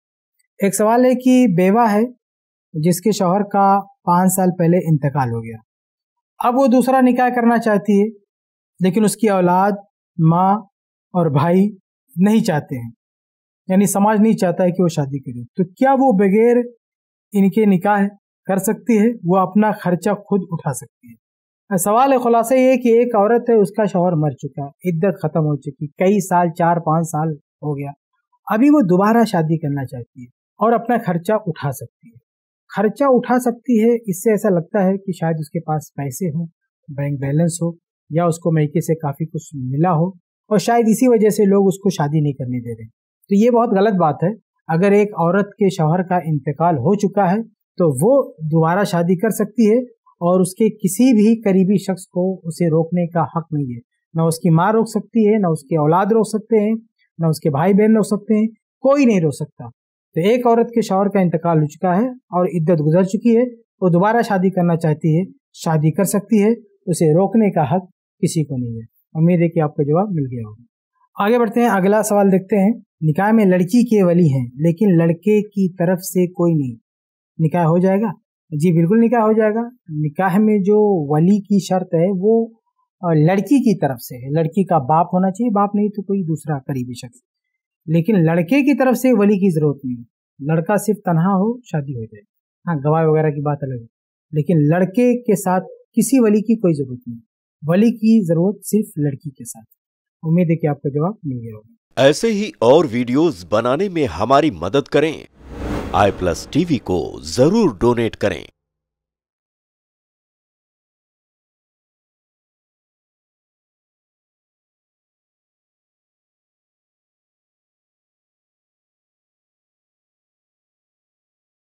Bewa Aurat Ke Mayke Wale Agar Use Nikah Karne Se Mana Karein Toh....? | Shaikh Kifayatullah Sanabili
Bewa Aurat Ke Mayke Wale Agar Use Nikah Karne Se Mana Karein Toh. ? | Shaikh Kifayatullah Sanabili | iPlus TV